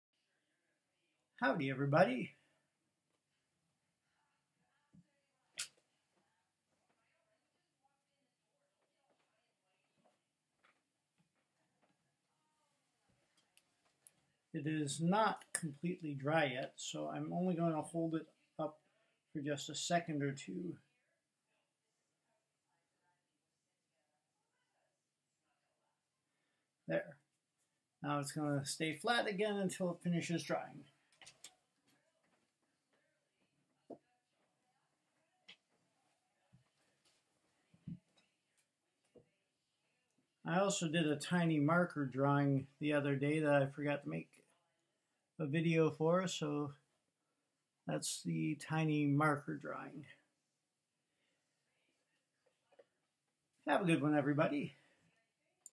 f'art reveal: not perfectly dry yet, and a tiny drawing
It's not perfectly dry yet, but dry enough to give a quick peek.  Also, I did a tiny drawing the other day that I hadn't put on video yet.  F'art on!